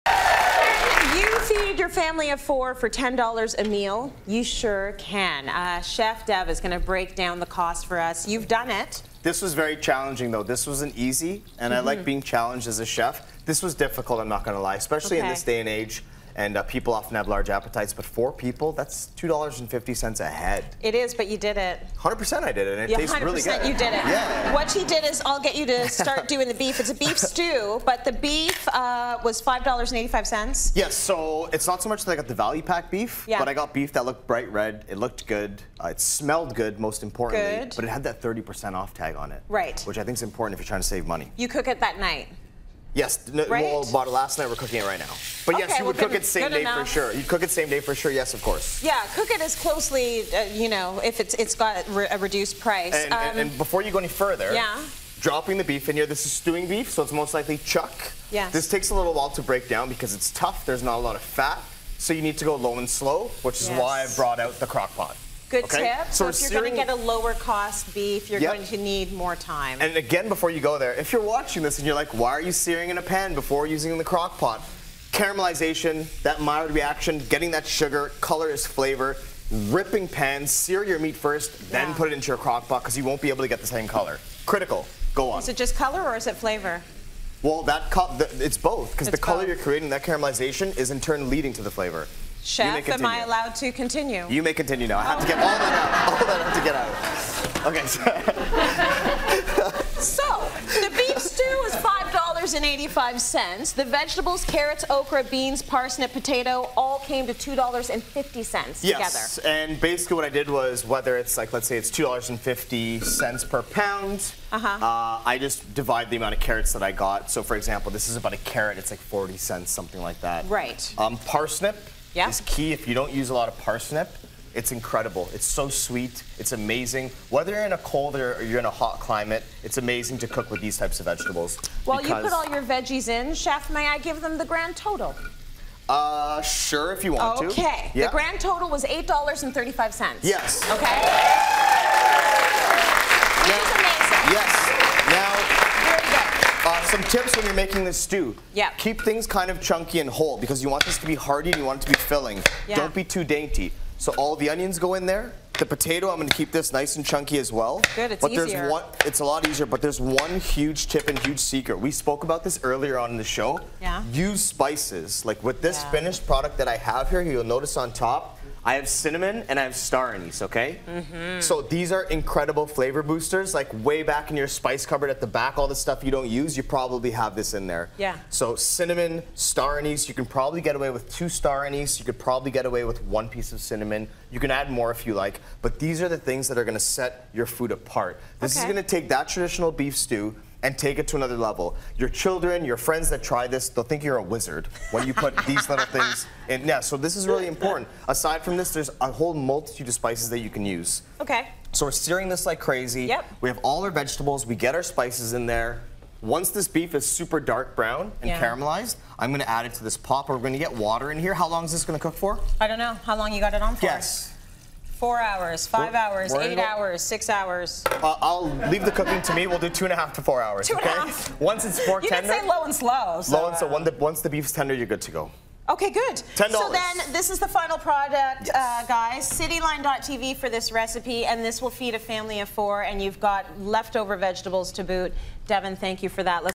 A beef stew for a family of 4 for only $10
Chef Devan Rajkumar guides us through a delicious meal to make for our family as such a low cost beef stew.